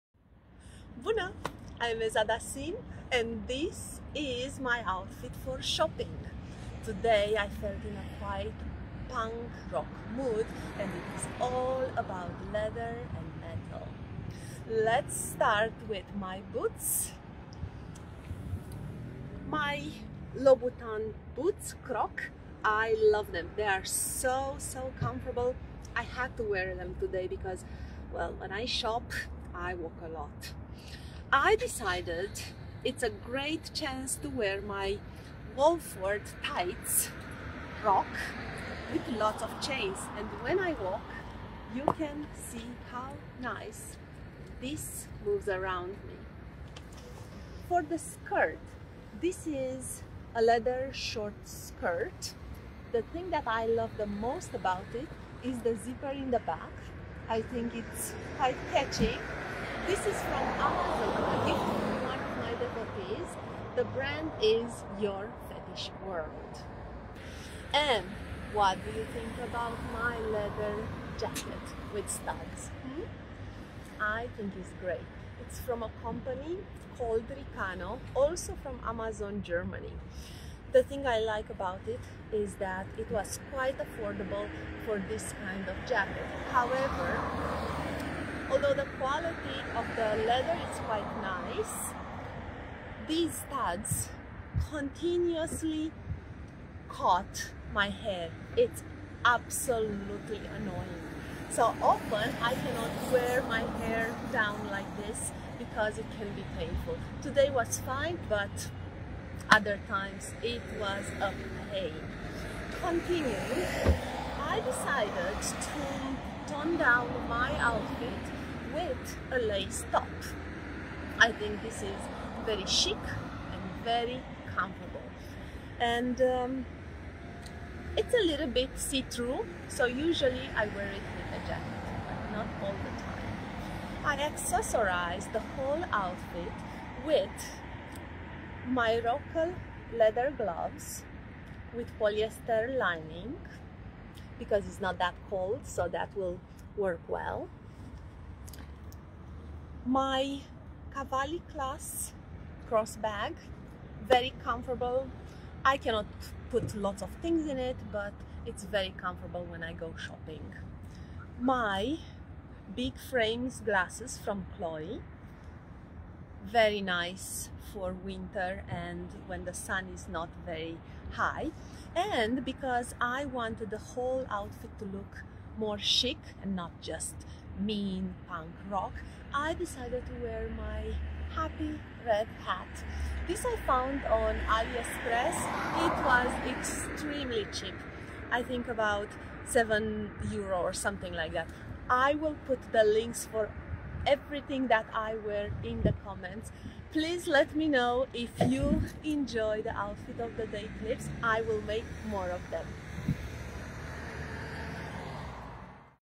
Shopping in Studded Leather [The Outfit of the Day]
For a day of shopping I decided for a punk rock look (black studded leather and tights with chains) tamed by a feminine lace top and a colorful hat.

How do you like this look?